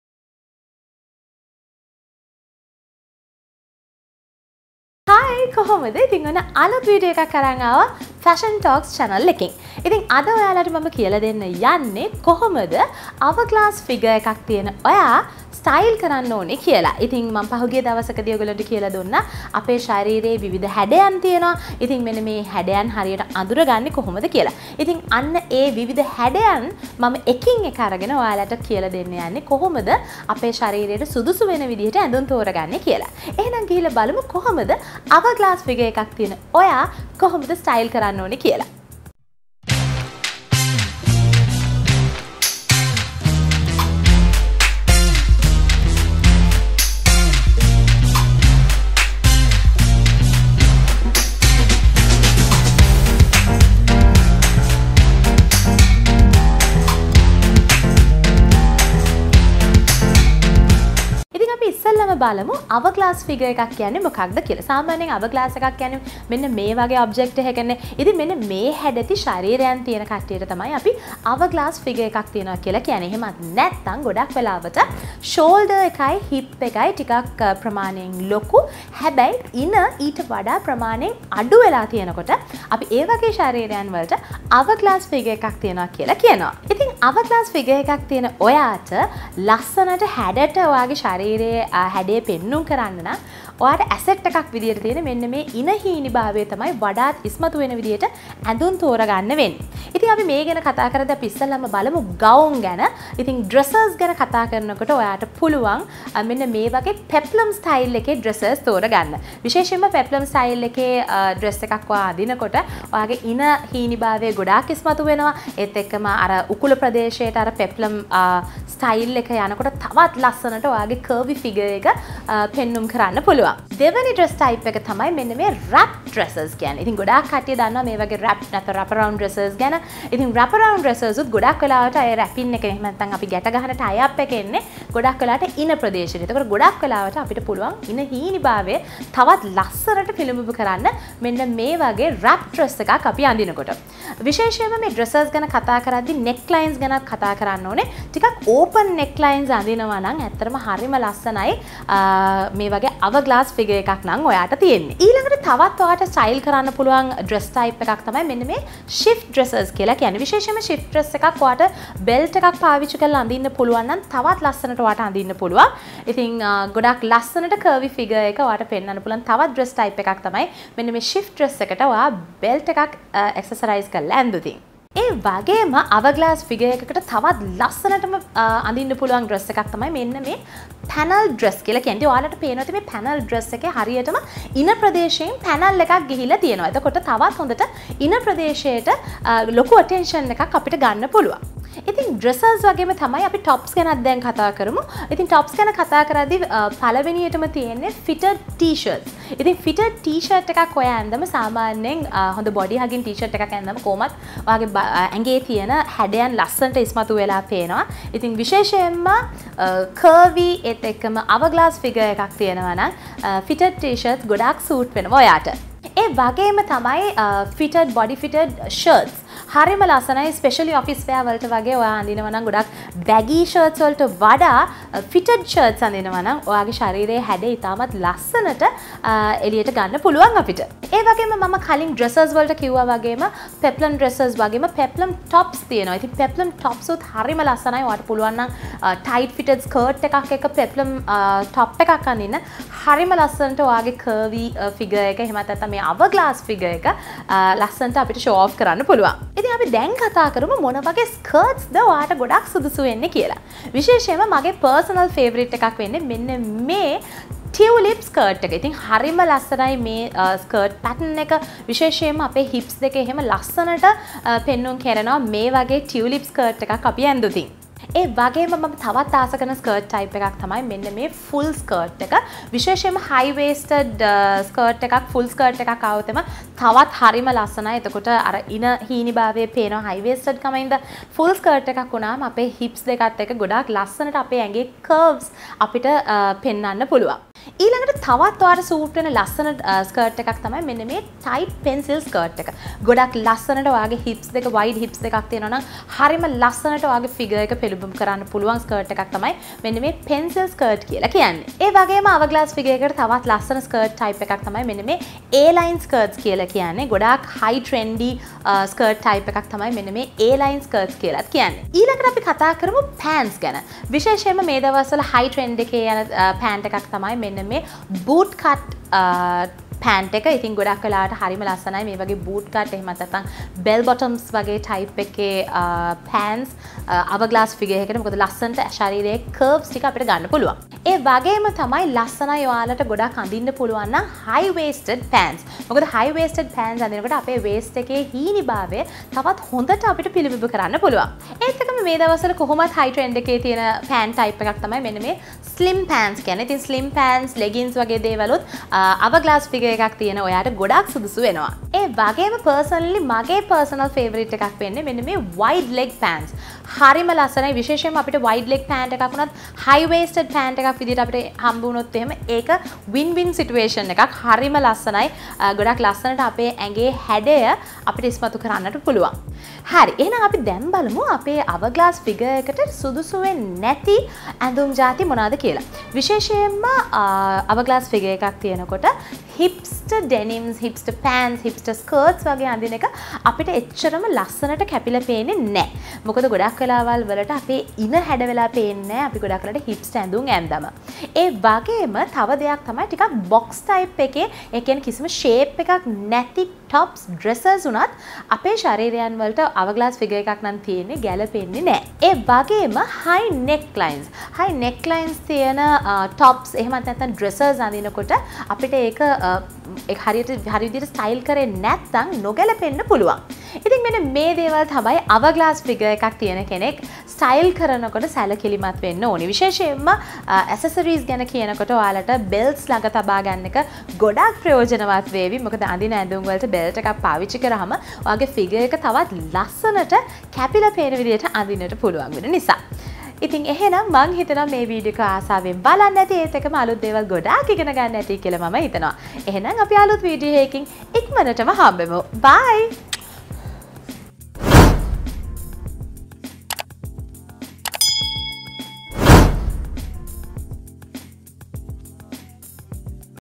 How To Style An Hourglass Body | Sinhala Styling tips 2021
Lets style an hourglass shape body. Hourglass shape is considered the ideal body shape so if you are thick or thin lets learn how to style it.